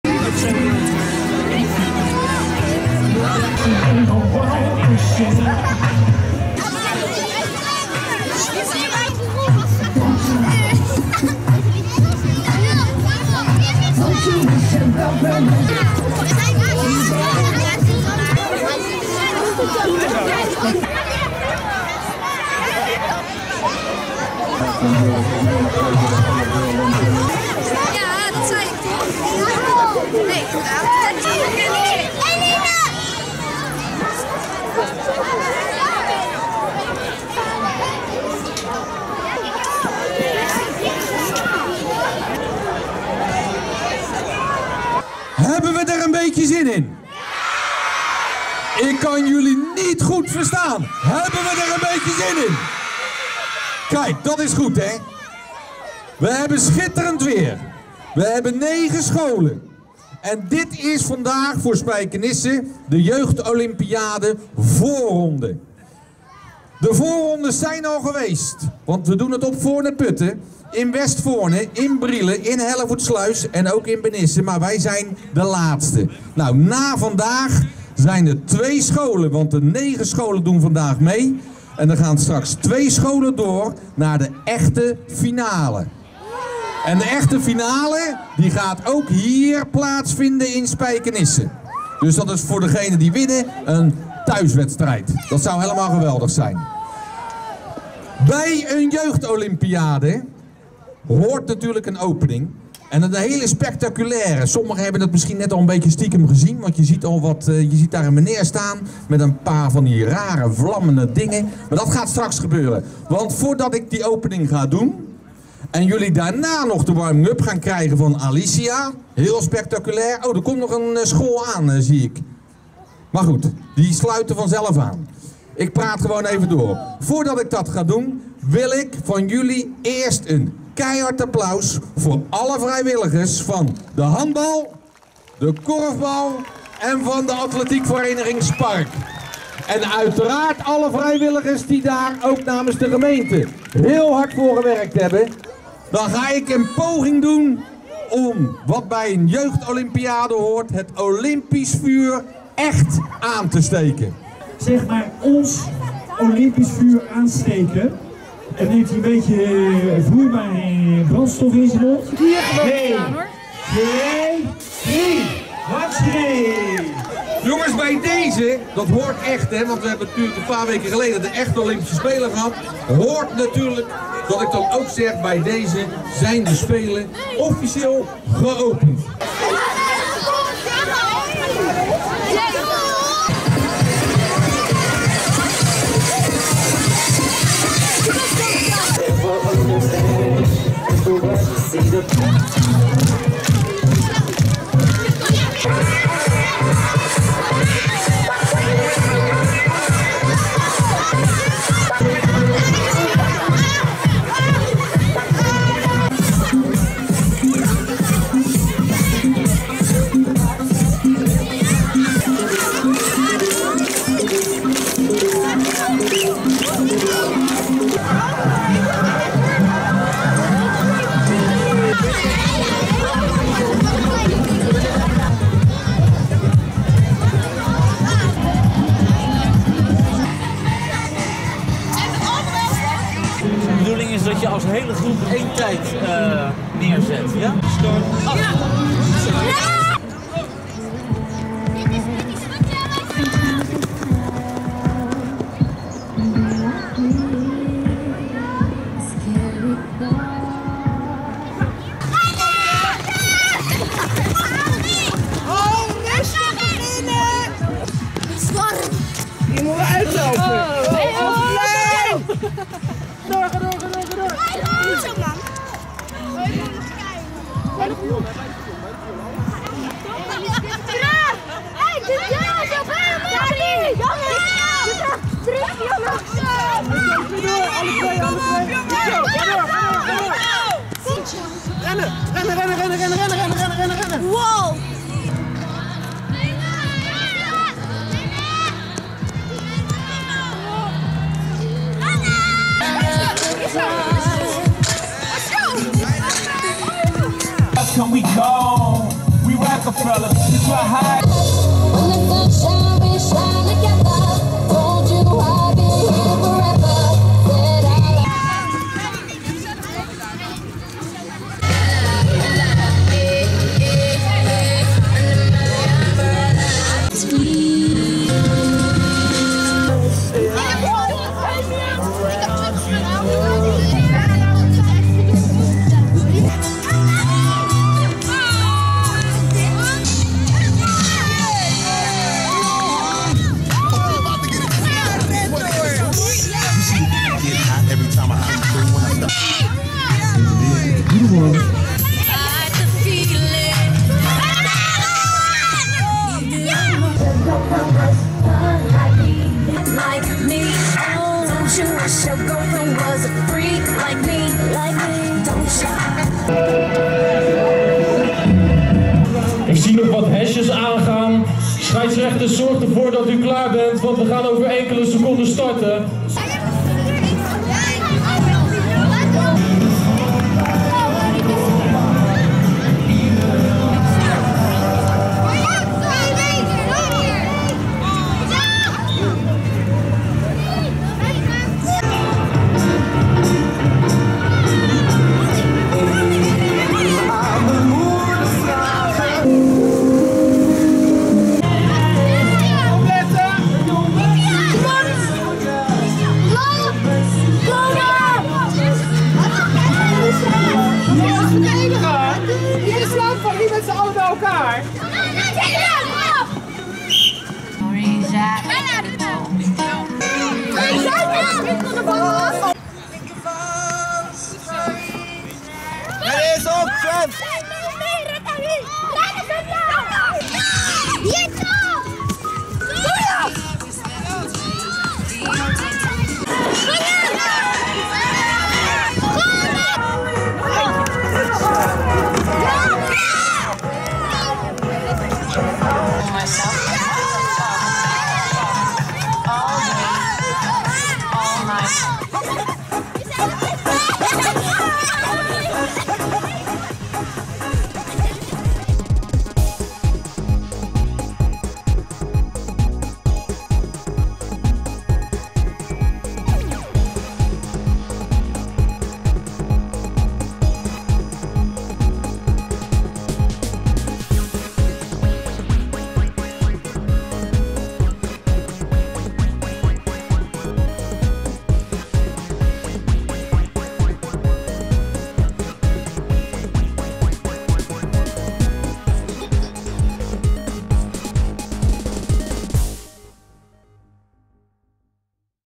Jeugd Olympiade / Spijkenisse 2014
Wethouder Jan Willem Mijnans opende dinsdag 25 maart om 09.00 uur de Jeugd Olympiade op het terrein van AV SPARK, sportpark De Brug. De wethouder stak het Olympisch vuur aan door vuur te  spuwen.
Basisschool leerlingen uit groep 6 en 7 streden tegen elkaar tijdens de  Jeugd Olympiade van Voorne-Putten. De vijf gemeenten op het eiland wil hiermee de jeugd aan het sporten krijgen en de samenwerking tussen de verenigingen en scholen bevorderen. Op heel Voorne & Putten zijn voorrondes gehouden. De beste teams strijden tegen elkaar in Spijkenisse.